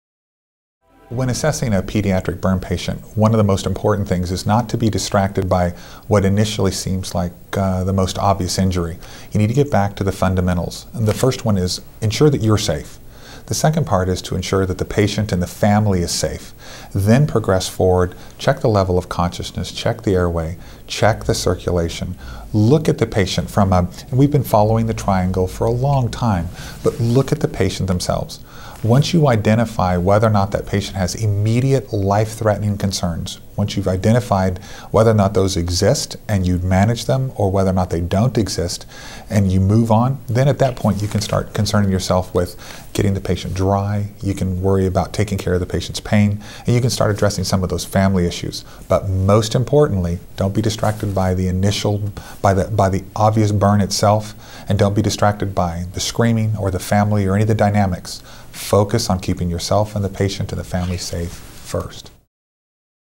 24-7 EMS: Pediatric Emergencies: Burns (Preview)
This course prepares EMS providers for the possibility of caring for and treating a pediatric burn patient through review of anatomy, current treatment theory, identifying appropriate hospitals, and includes coaching on how to mentally prepare for the child and family. Featuring Scotty Bolleter, BS, EMT-P Director of Education San Antonio AirLife San Antonio, Texas